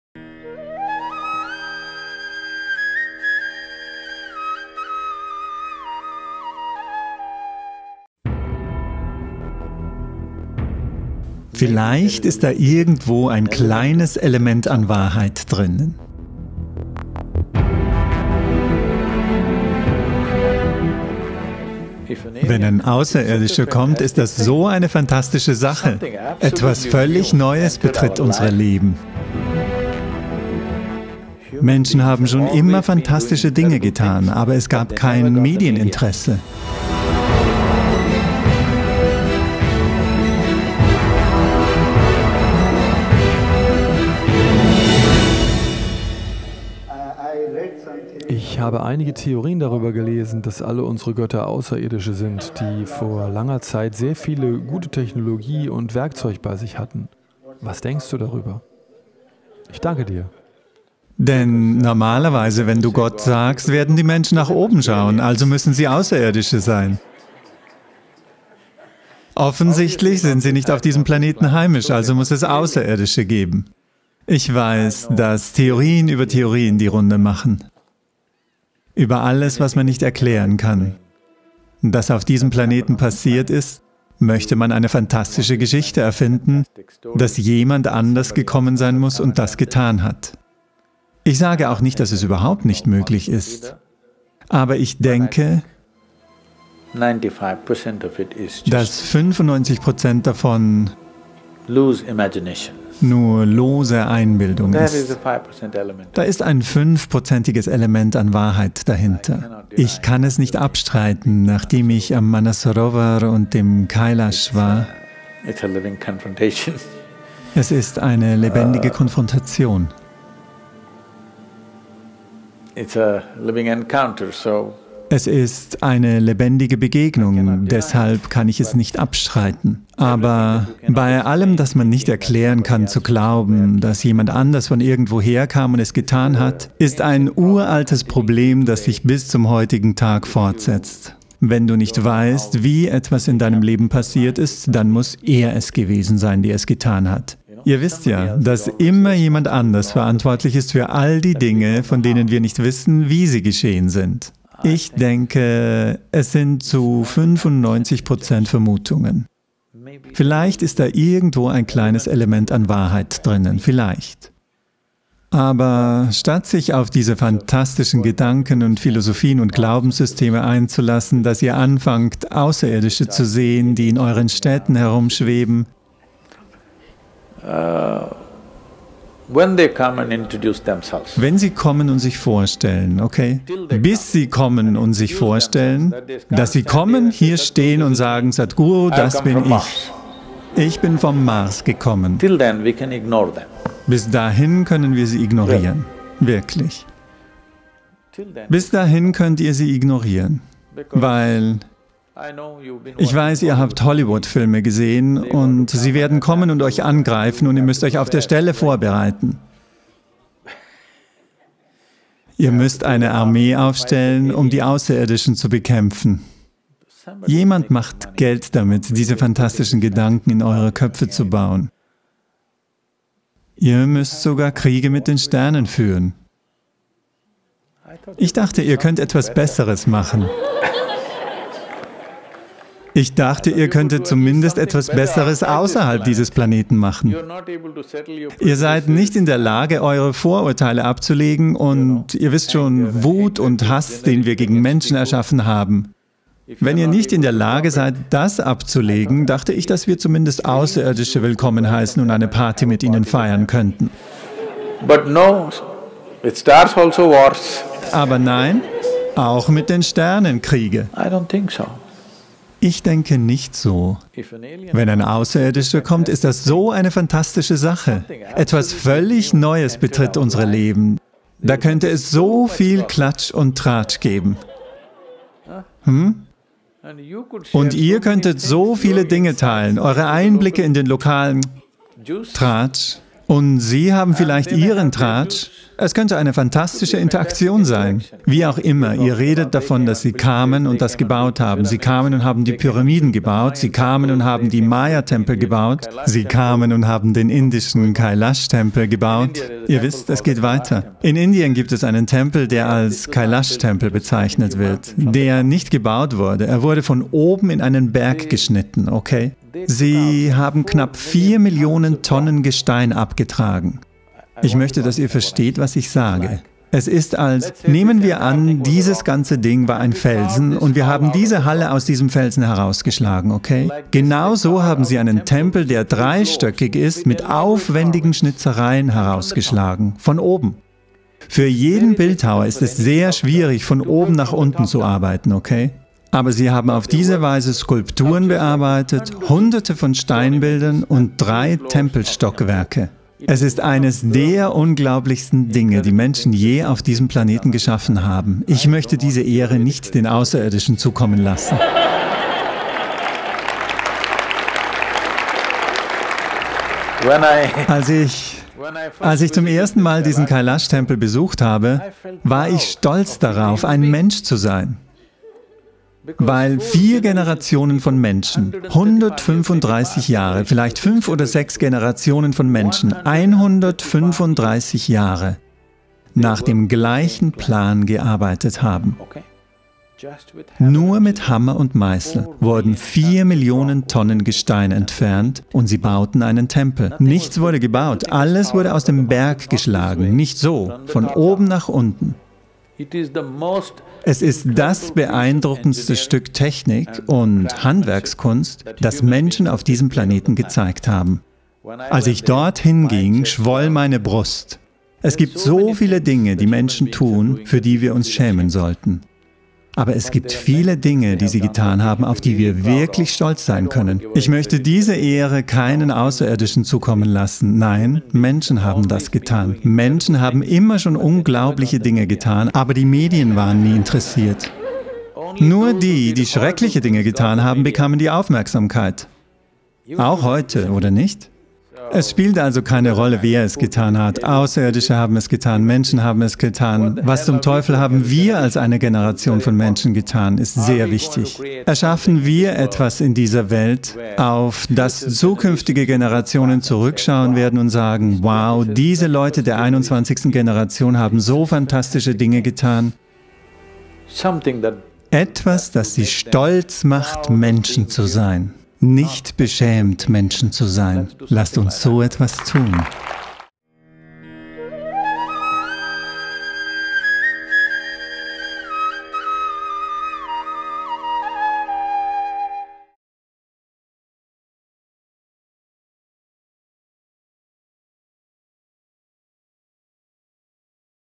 Sind unsere Götter eigentlich Außerirdische? | Sadhguru
Sadhguru beantwortet die Frage, ob diejenigen, die wir Götter nennen, tatsächlich Außerirdische mit besserer Technologie und besseren Werkzeugen von einem anderen Planeten sind. Siehe Sadhgurus Antwort, die gleichzeitig leichtherzig und aufschlussreich ist.

Mehr Videos & Blogs auf der Webseite